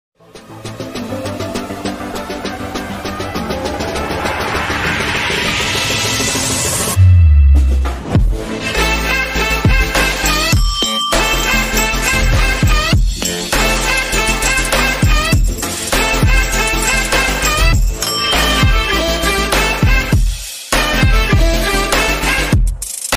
• 2bhk 724sqft carpet | Ready To Move With OC
• It's a big complex
 22storey Tower

• Amenities - Swimming pool , gym , mini theatre , kid play area

• 2bhk 724sqft carpet area
Ready to move
• Price 1.55cr flat cost

• Location Jankalyan nagar Malad west